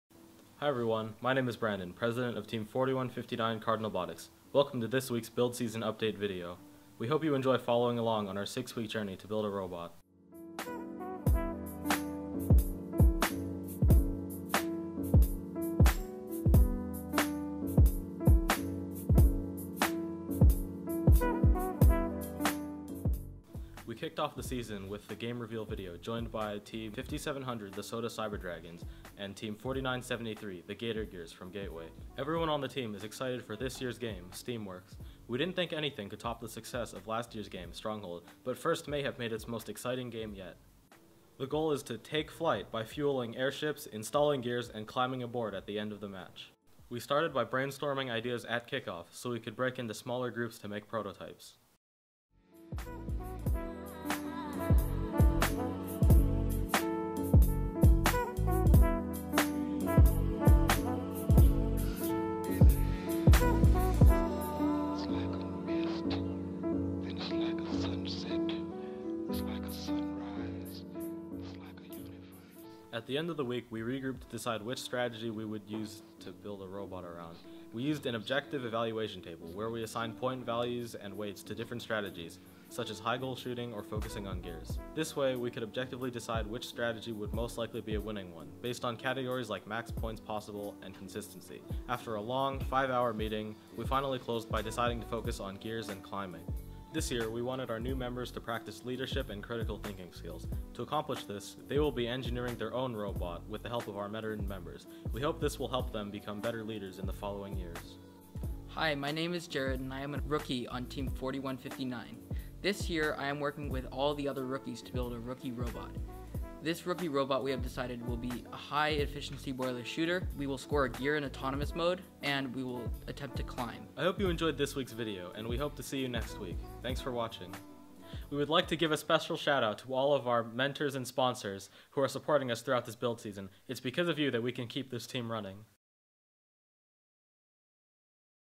Team 4159 CardinalBotics Weekly Updates Week 1
Hello we are team 4159 Cardinalbotics and welcome to our weekly update videos for the 2017 FRC, Steamworks. We hope you enjoy our progress towards building our robot for 6 weeks.